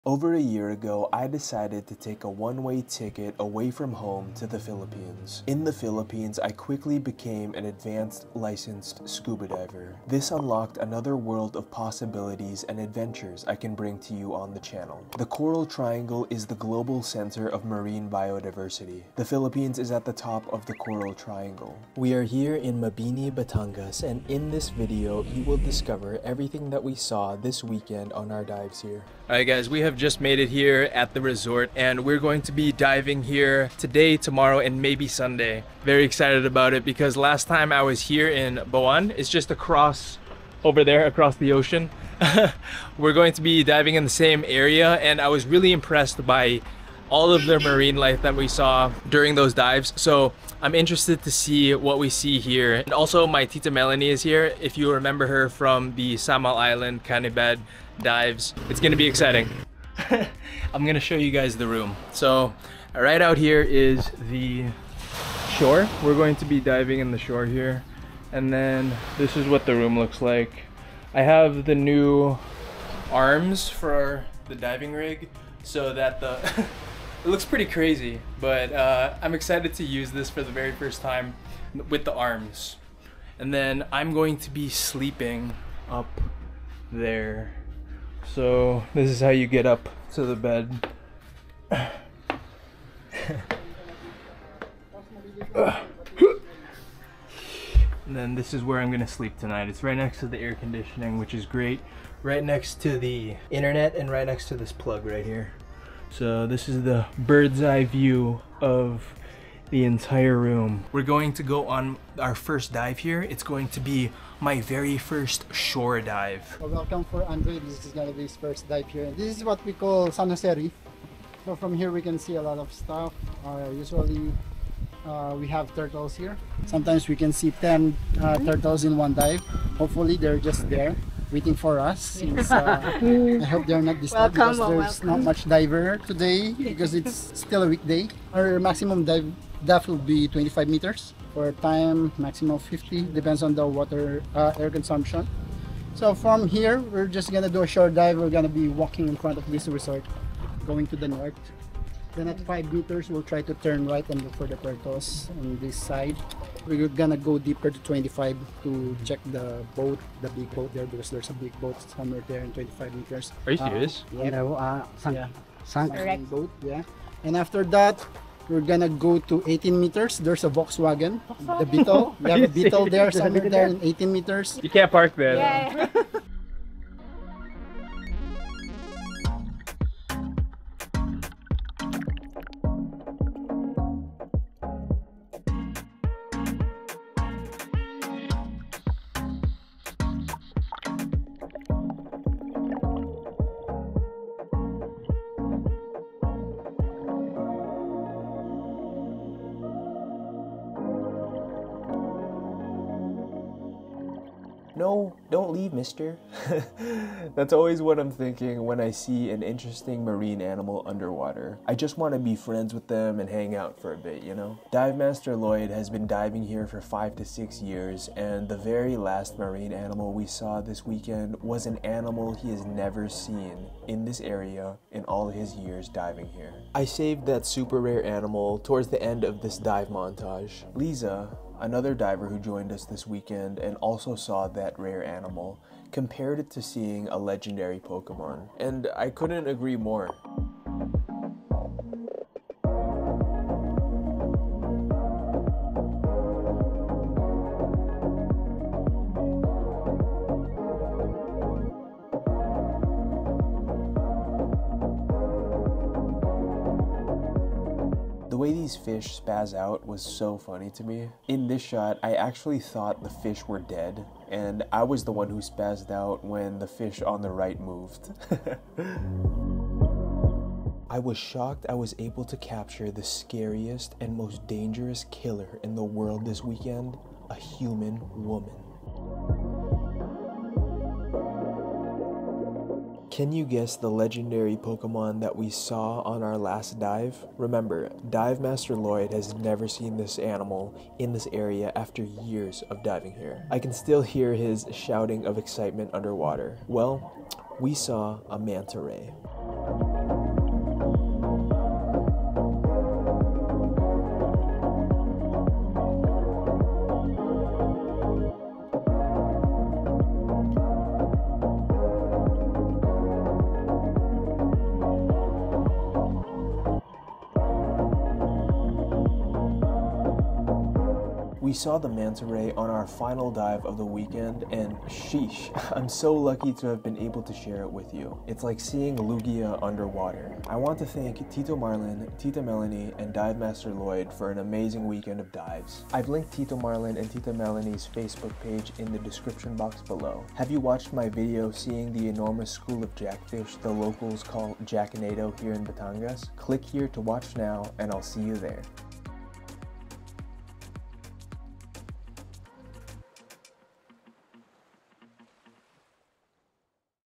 ULTRA RARE Fish Encounter (Scuba Diving)
In this video we are visiting Mabini, Batangas Philippines for the weekend to scuba dive.

This is a compilation of all 6 scuba diving we went on during the weekend.

On our last dive we discovered a fish that has not been seen in this area even by the Dive Master who has been diving here for 5-6 years!

↗️ Share the video if you think someone else can learn and enjoy this video.

Timestamps
00:00:00 Welcome To The Video!
00:00:59 Room Tour & Pre-Dive Briefing!
00:02:56 Time To Dive!
00:05:30 Thank You!